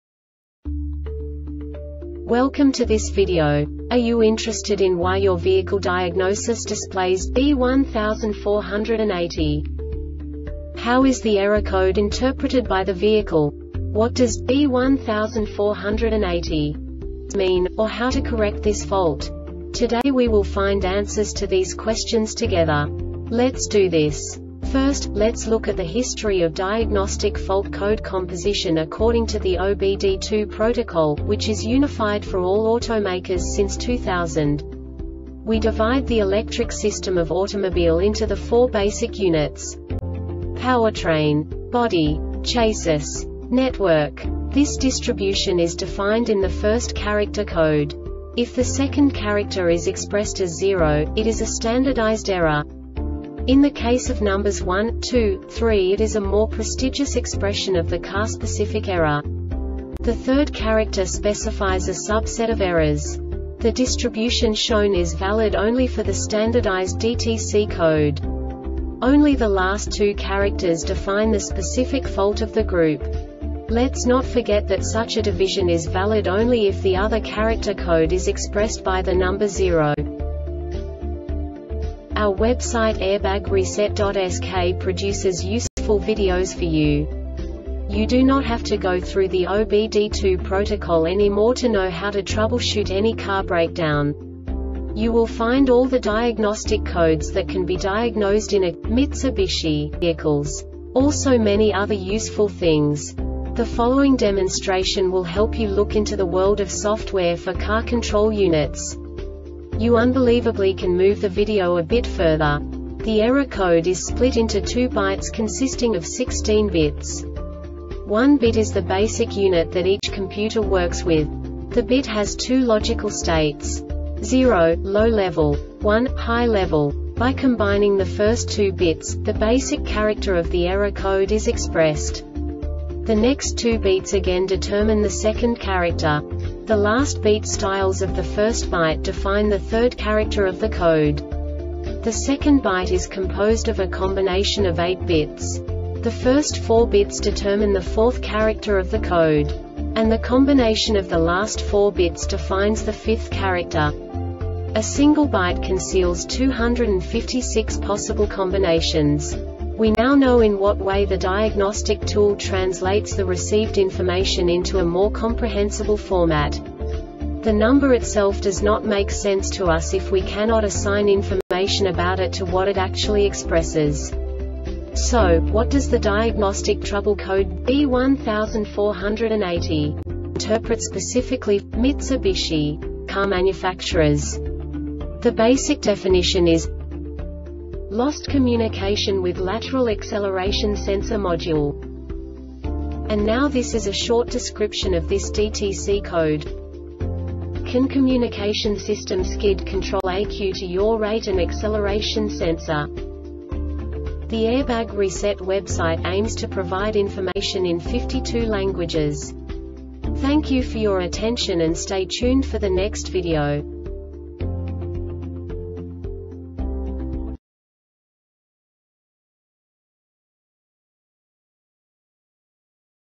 DTC Mitsubishi B1480 Short Explanation
Contents:
0:21 Basic DTC analysis according to OBD2 protocol standard.
1:48 Insight into programming
2:58 A diagnostically understandable description of the Diagnostic Trouble Code
3:05 Basic error definition

Make:
Mitsubishi
Code:
B1480
Definition:
Lost Communication with Lateral Acceleration Sensor Module
Cause: